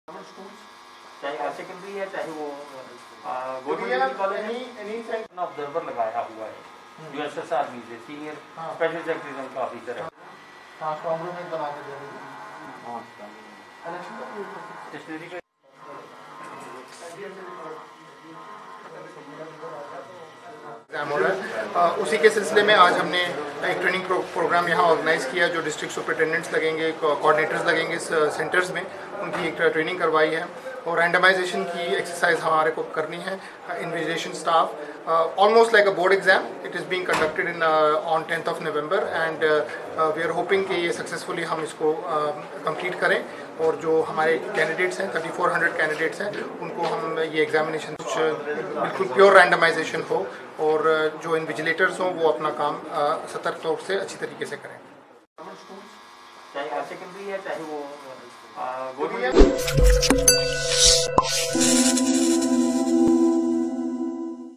DDC Ramban reviews arrangements for AA Panchayat exam, presides over training program
District Development Commissioner, Nazim Zai Khan, today presided over the training programme organized to impart Accounts Assistant Panchayat Examination related guidance to designated District Superintendents and Coordinators.

He also reviewed the arrangements being made by the different departments for smooth conduct of OMR based objective type written examination for Accounts Assistant (AA), Panchayat being conducted by J&K Service Selection Board.

            Additional Deputy Commissioner (ADC), Ashok Sharma; ACR,   Dhirender Sharmam;  CEO, Vinod Koul; Dy.SP, Headquarter, ARTO, Principals of various institutions and representatives of government and private institutions attended the meeting.

At the outset, the ADC was informed that the examination shall be held on November 10, (Tuesday) from 12 noon to 2 pm at 28 examination centers in the district.

It was decided that Superintendents, Deputy Superintendents, and supervisors shall be engaged for the purpose, besides required invigilators as per the requirement of one invigilator for 24 students through randomizations, while Magistrates shall also be appointed at all 28 examination centers. ‘It was also informed that videograpy of the examination process would be done in all 28 examination centers.

             The Chief Education Officer was directed to provide a list of 200 teachers for deploying as invigilators after randomization.

The Assistant Regional Transport Officer was directed to provide vehicles for ferrying of examination papers from the strong room to the examination center. He was also asked to submit a transport plan immediately.

The Health authorities were directed to ensure implementation of SPOs issued by the government to prevent COVID-19 infections at examination centers.

The aspirants would not be allowed to carry  mobile phones and other electronic devices inside the exam center.